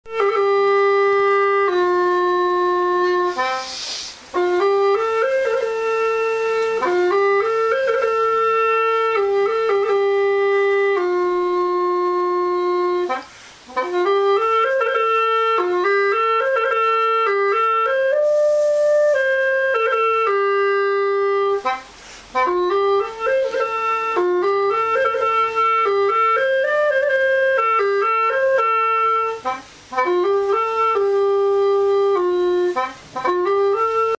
CLEMATRIX JAZZMAN ETHNIC -VOYAGE MUSICAL - DECOUVERTE "J'IRAI JOUER CHEZ VOUS " MUSIQUE D'UN MONDE
Clematrix  improvise  avec cette flûte d'un autre monde à anche libre au son de la clarinette du nord-est  de la Thailande assez répandu dans tout le pays qui date de l'âge du bronze !